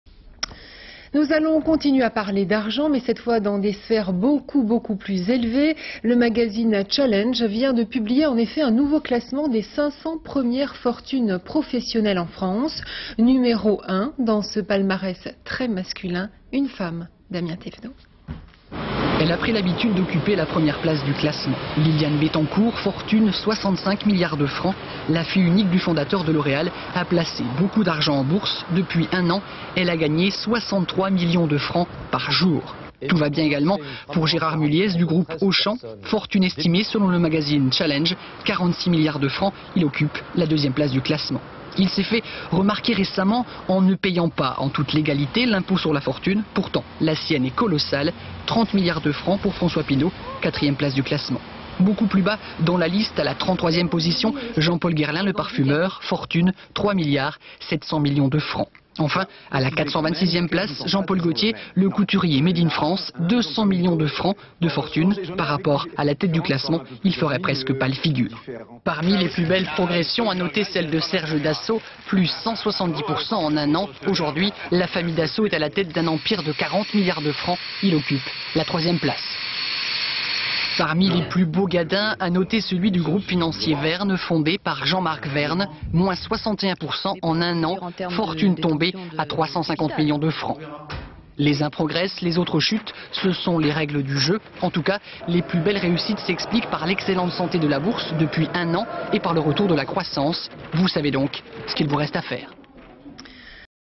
Le classement des plus grandes fortunes françaises
24 juin 1998
SOCIETE : Le magazine CHALLENGE a publié le classement des plus grosses fortunes de France avec toujours en tête Liliane BETTENCOURT propriétaire de l'Oréal. Images avec encadré.       - Photo de Liliane BETTENCOURT, l'Oréal. Images en encadré des rayonnages de boins de beauté et autres produits l'Oréal.       - 2ème : Gérard MULLIEZ, président du groupe Auchan. Images en encadré.       - 4ème François PINAULT, Le Printemps, La Redoute. Images encadré.       - 33ème Jean-Paul GUERLAIN, parfumeur testant une de ses création.       - 426ème Jean-Paul GAULTIER, images encadré, interview       - A la 3ème place, Serge DASSAULT. Images encadré cockpit avion de guerre.       - Le gadin : la famille VERNES de VERNES-Finances chutant à la 295ème place. Images encadré, femmes comptant les planches de billets de banques. Image de la corbeille de la Bourse.       - [Gros plan] Billet de 100Frs. Images d'archive INA